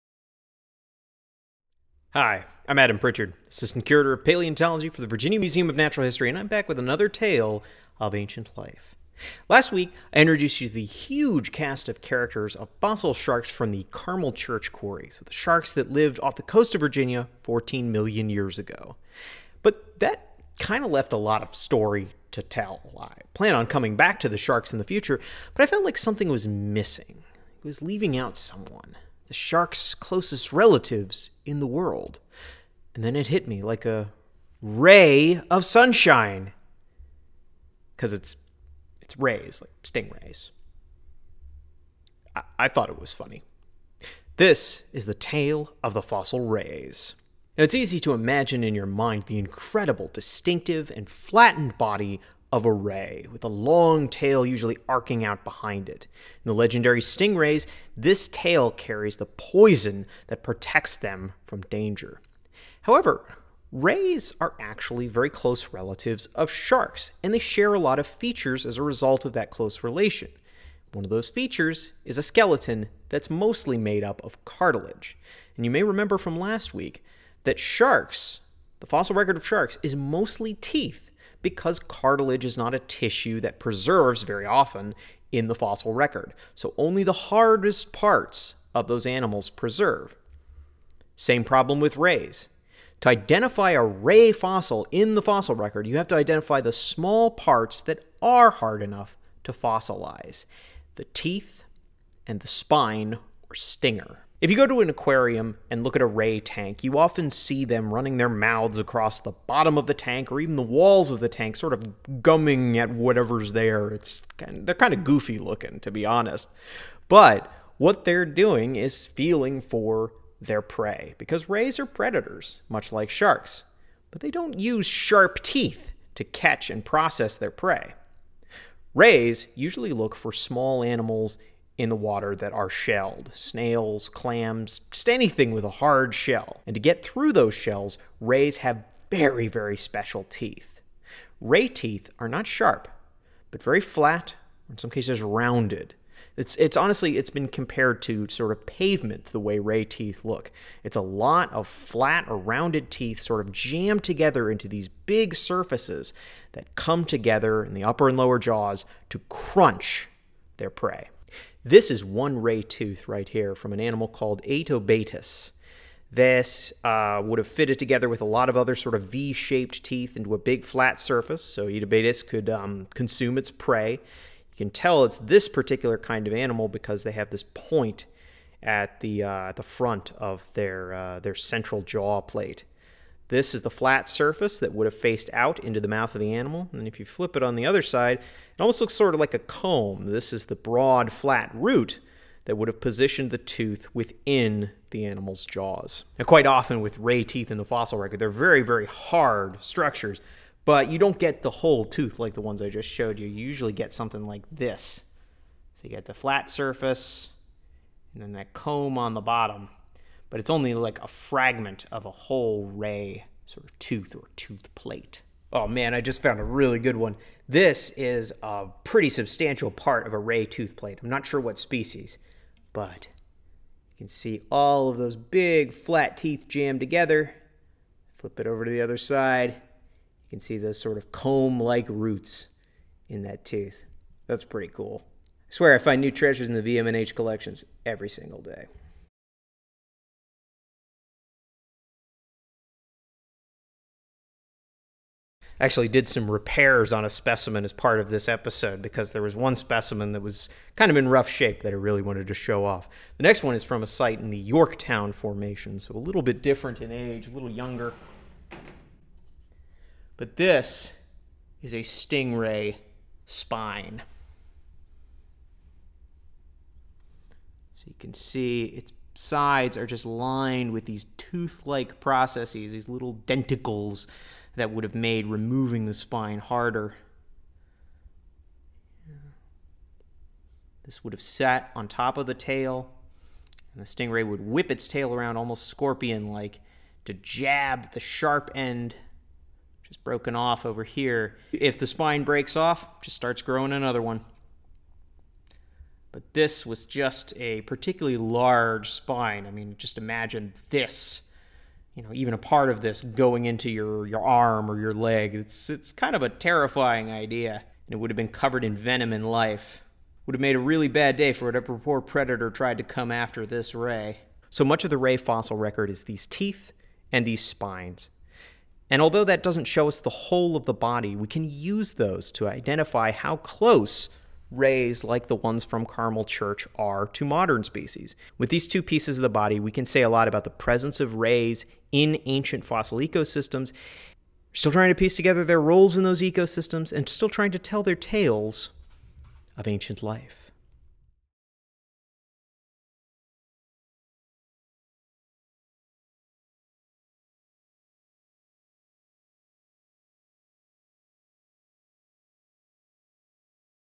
Tales of Ancient Life: The Tale of Fossil Rays
Assistant Curator of Paleontology Dr. Adam Pritchard continues putting the spotlight on ancient marine animals this week by highlighting fossil rays in his latest episode of "Tales of Ancient Life"!